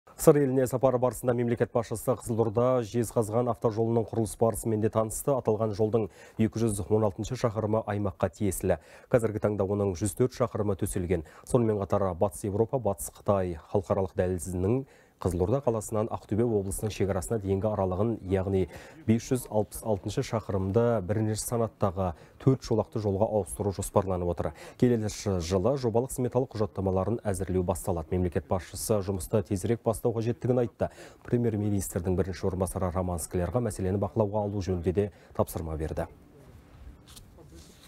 Қ.Тоқаев Қызылорда – Жезқазған автожолының құрылыс барысымен танысты
Сыр еліне сапары барысында мемлекет басшысы Қызылорда – Жезқазған автожолының құрылыс барысымен де танысты.